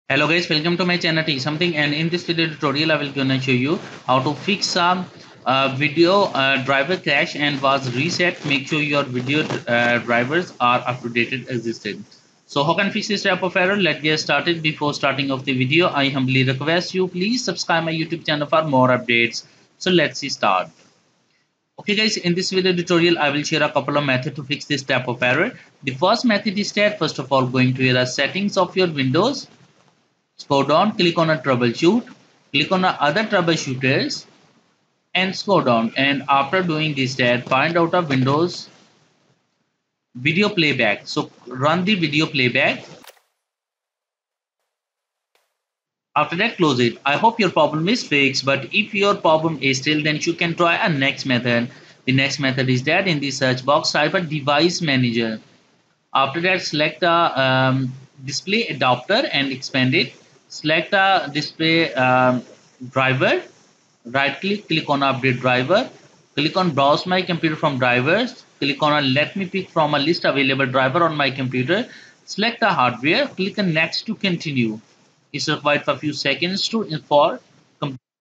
Fix Video Driver Crashed And Was Reset! Make Sure Your Video Drivers Are Up To Date Exiting
In this video tutorial, we will show you How to Fix Video Driver Crashed And Was Reset! Make Sure Your Video Drivers Are Up To Date Exiting.